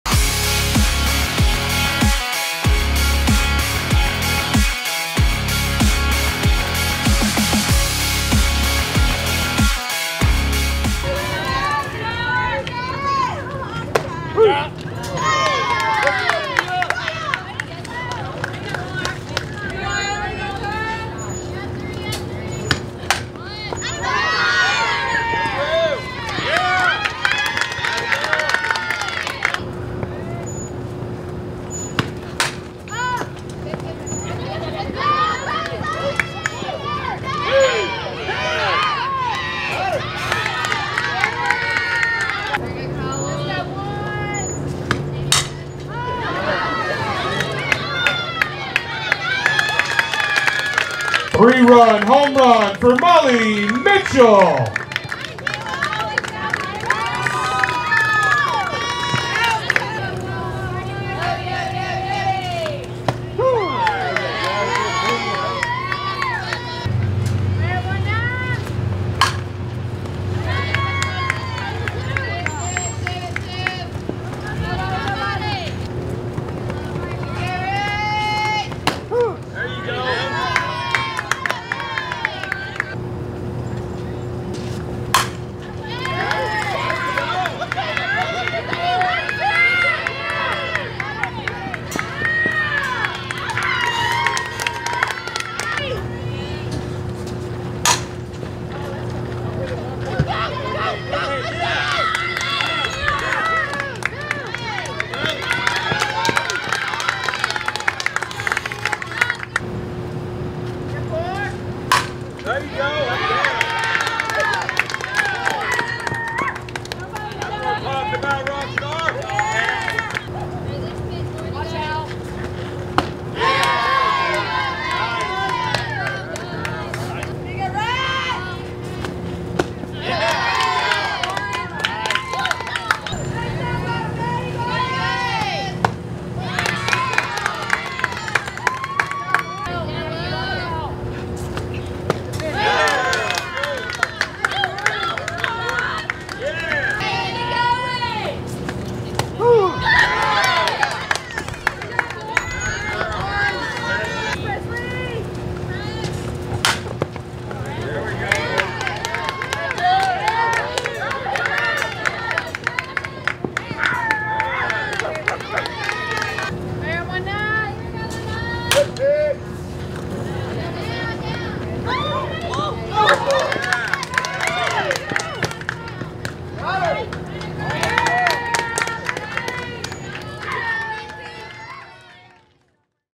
MCC: Softball vs Bevill State Highlights
Highlights of the Lady Eagles games against Bevill State on February 27, 2024, including a shutout victory in game two.